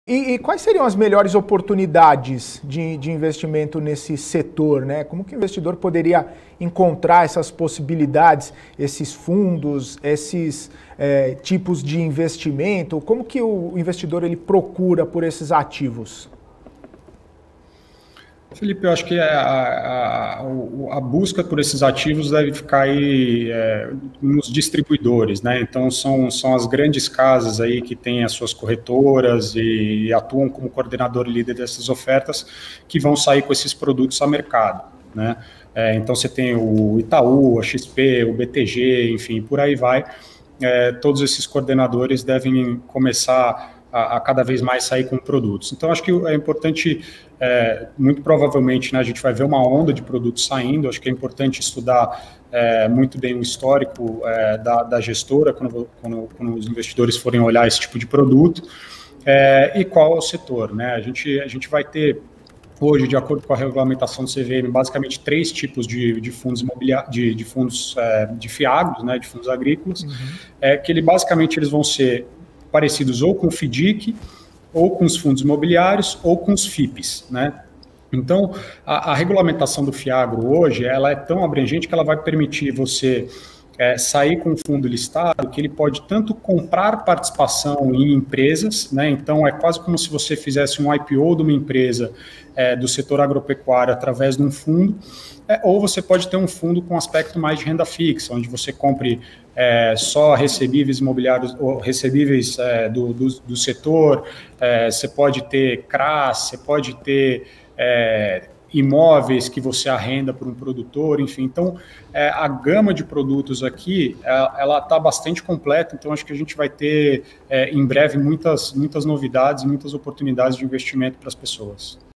QUAIS OS MELHORES FUNDOS AGRÍCOLAS PARA INVESTIR? MUCIO MATTOS AVALIA
Mucio Mattos, sócio e head de crédito da Vectis, avalia quais são os fundos agrícolas que mais se destacam no cenário atual para investimentos.